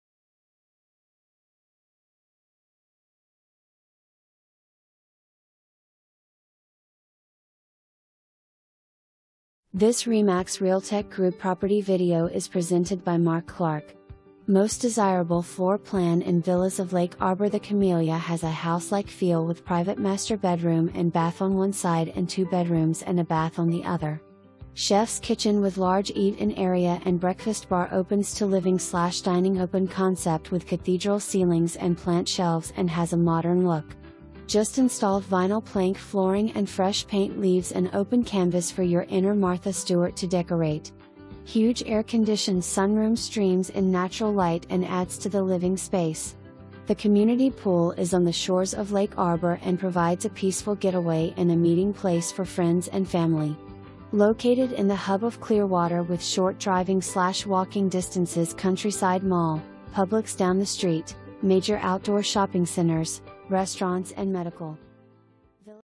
RE/MAX Realtec Group - 2242 BLOSSOM WAY
Please enjoy this property video created by RE/MAX Realtec Group

2242 BLOSSOM WAY
$245,000, 3 bed, 2.00 bath, 1,670 SF, MLS# U8089387

MOST desirable floor plan in Villas of Lake Arbor the Camellia has a "HOUSE LIKE" feel with private master bedroom and bath on one side and 2 bedrooms and a bath on the other. Chef's kitchen with large eat-in area and breakfast bar opens to living/dining open concept with cathedral ceilings and plant shelves and has a modern look. Just installed vinyl plank flooring and fresh paint leaves an open canvas for your inner Martha Stewart to decorate. HUGE air conditioned sunroom streams in natural light and adds to the living space. The Community pool is on the shores of Lake Arbor and provides a peaceful getaway and a meeting place for friends and family. Located in the hub of Clearwater with short driving/walking distances Countryside Mall...Publix down the street..Major outdoor shopping centers,restaurants and medical.
Villas of Lake Arbor's motto is where neighbors become friends and it is so true!

2 full bath

Mark Clark
RE/MAX Realtec Group